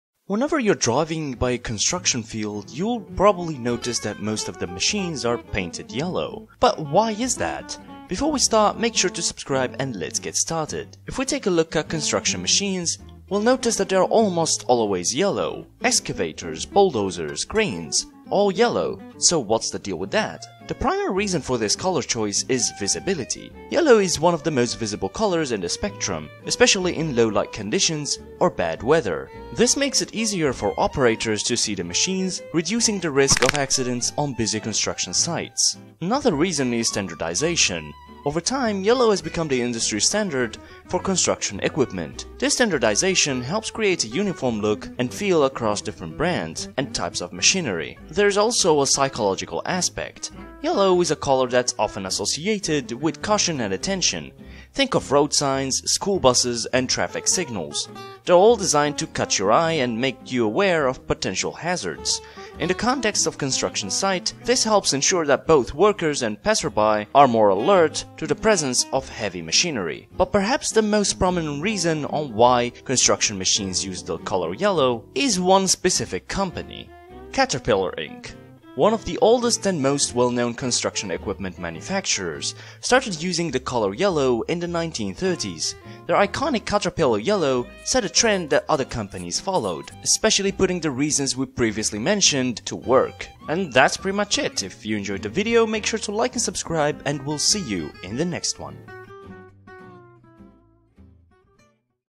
Why are Construction Machines Yellow?
Construction machines are painted Yellow, But ever wondered why that's the case? Today we'll find out!